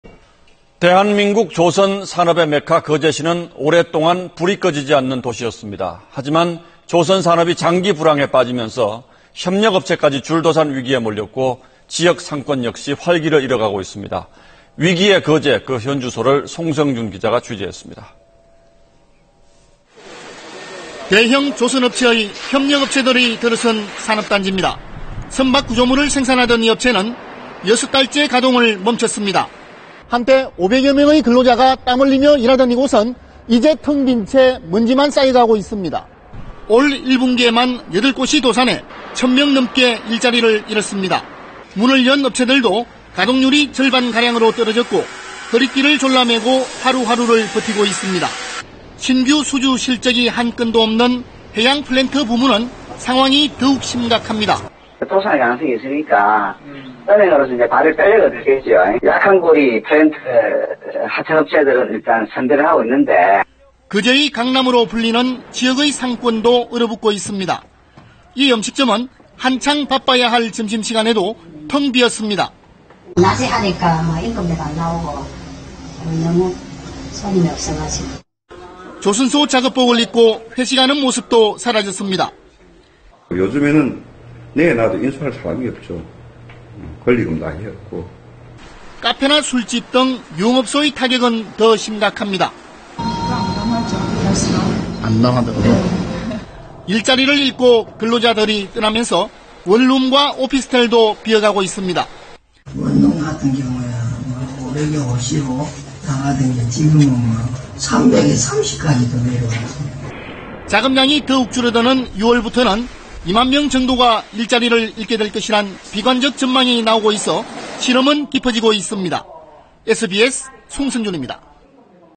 돈 넘쳐나던 '조선 메카' 옛말…사라진 근로자들 / SBS
대한민국 조선 산업의 메카 거제시는 오랫동안 불이 꺼지지 않는 도시였습니다. 하지만 조선 산업이 장기 불황에 빠지면서 협력업체까지 줄도산 위기에 몰렸고 지역 상권 역시 활기를 잃어가고 있습니다.